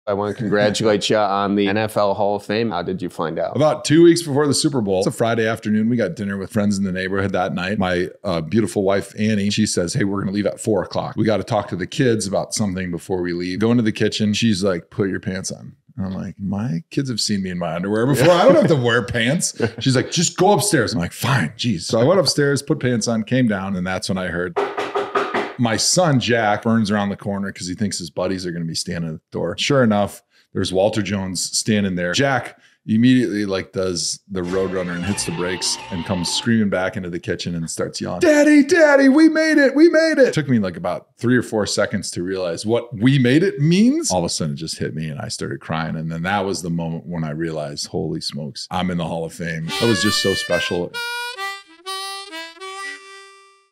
Joe Thomas Finding out He's in the Hall of Fame
We made it 🏆